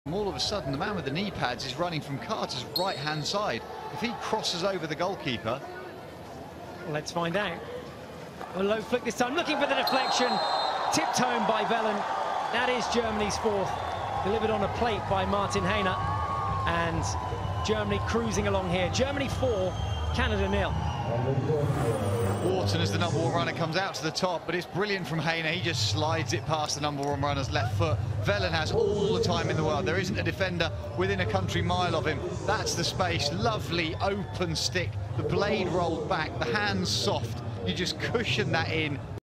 GER 4-0 CAN Wellen deflects the ball in from a well worked PC
Watch clips from the Germany - Canada game at the FIH 2015 Men’s Hero Hockey World League, taking place in Raipur, India.

The FIH Men’s Hero Hockey World League Finals take place from November 27th-December 6th, 2015, see the full schedule or check results here: